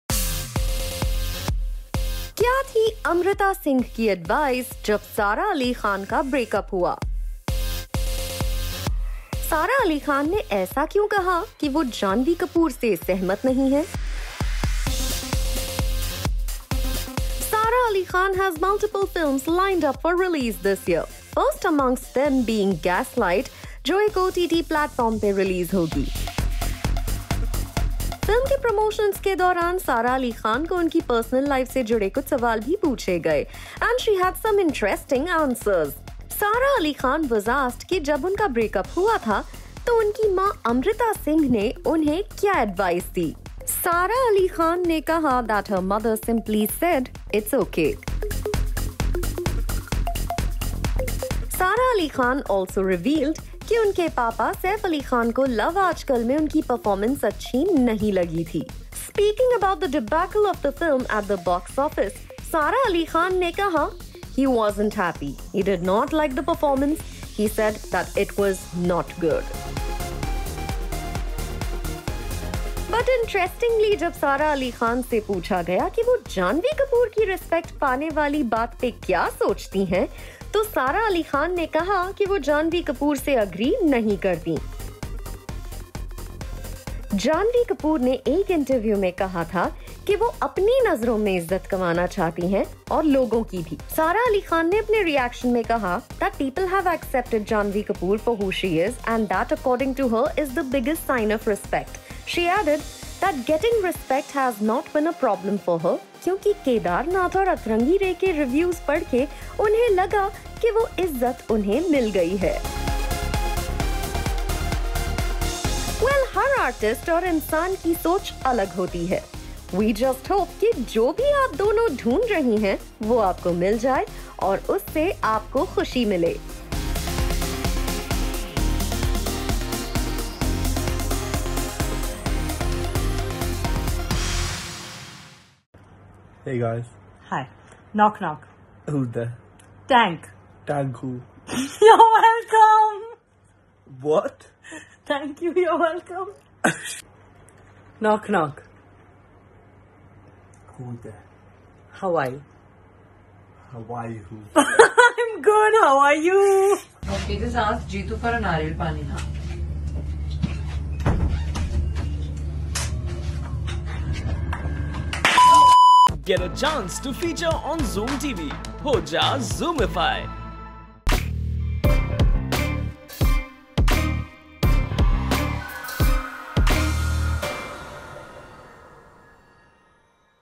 Sara Ali Khan REVEALS her mother Amrita Singh's reaction to her breakup
Sara Ali Khan is one of the most popular actresses in the industry and the actress recently revealed that this was her mother's advice when she came to know about her break up. Furthermore, Sara also added that her father Saif did not like her performance in 'Love Aaj Kal'. The actress also opened up about Janhvi Kapoor's respect statement. Check out the video for more!